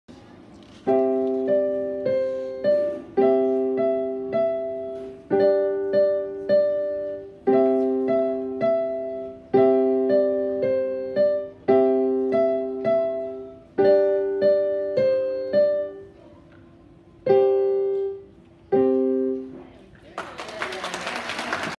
Year 1&2 Assembly Jan’19
Reem 2C playing the piano